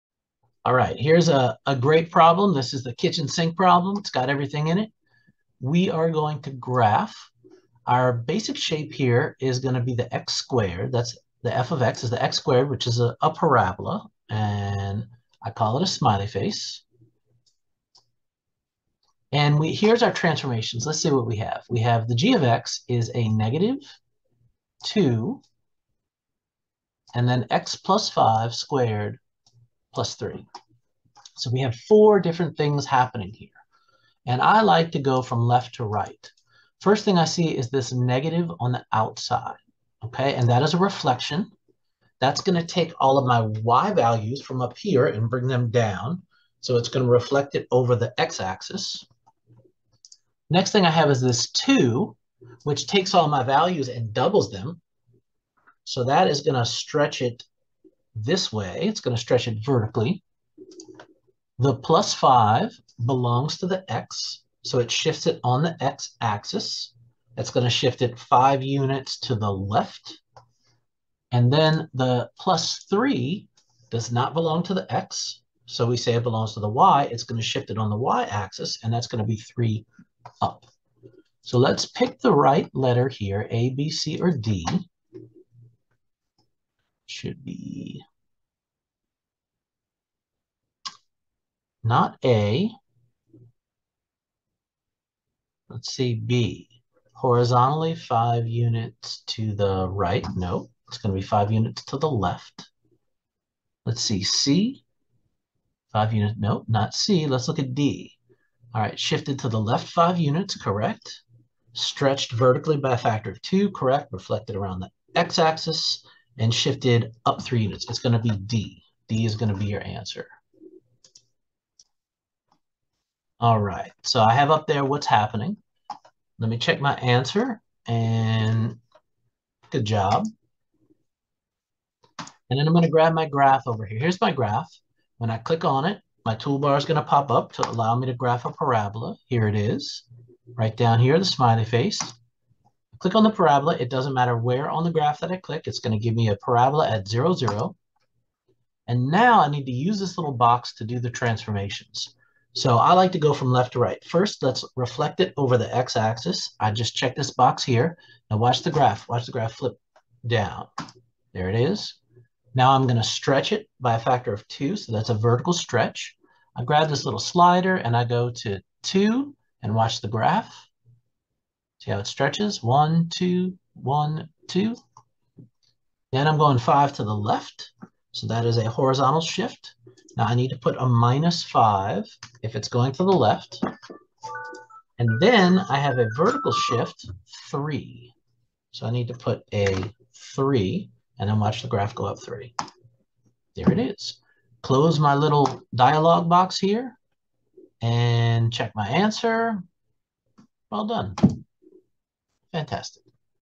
1.6.65 Graph the Transformation of a Parabola in Pearson's MyLabMath
1.6.65 Graph the Transformation of a Parabola in Pearson's MyLabMath
Reflect, Stretch, Shift
College Algebra / PreCalculus
Professor McMath
ProfessorMath1234